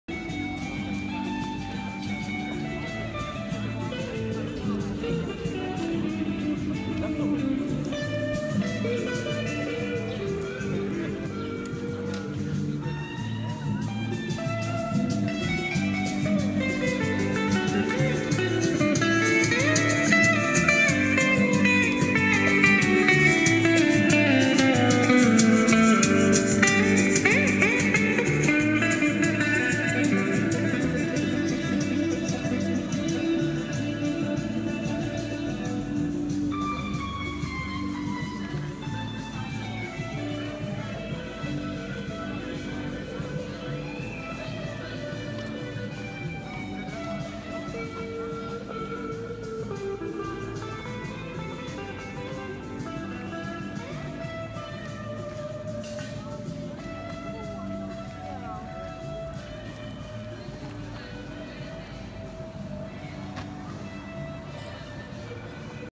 Where in the World is Louise? Berlin :: James Simon Park
James Simon Park Berlin Germany - beautiful day for a stroll in a beautiful part - check out my blog posts